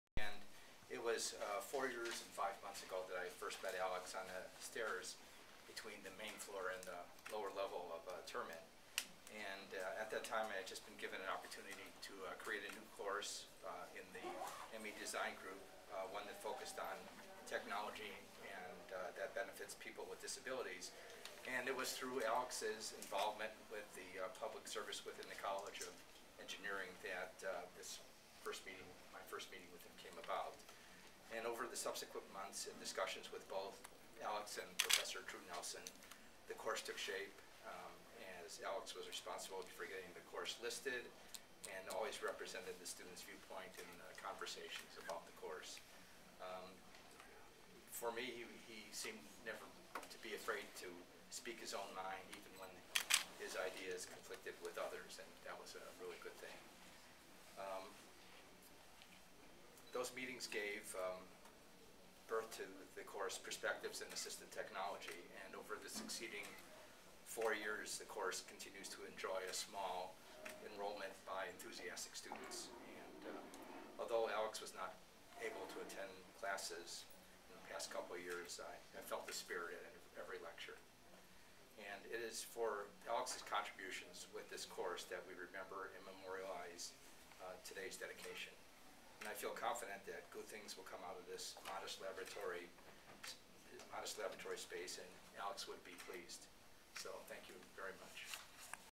03 David Jaffe
Lab room dedication service for Alex Tung.
David Jaffe - Associate Director, Assistive Technology Laboratory at Stanford (ATLAS)